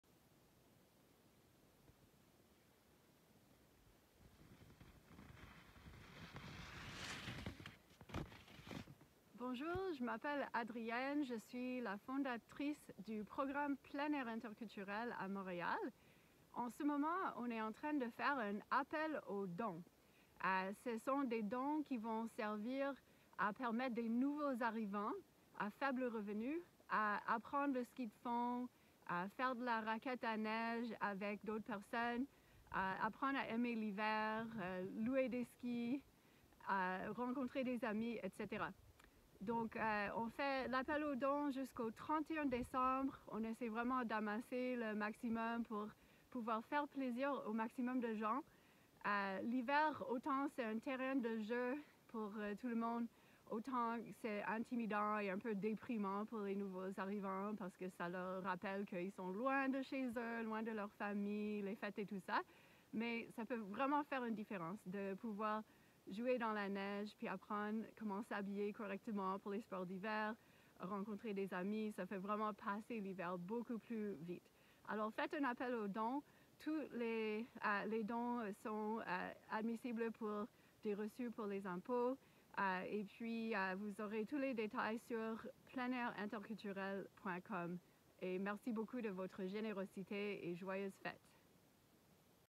Offrez les joies de l'hiver! Offer the Joys of Winter!
Faites un don d'ici le 31 décembre afin de parrainer la participation à des nouveaux arrivants à faible revenu à nos initiations au ski de fond et à la raquette! 31st to sponsor the participation of low-income newcomers to Canada in our cross-country skiing and snowshoeing outings!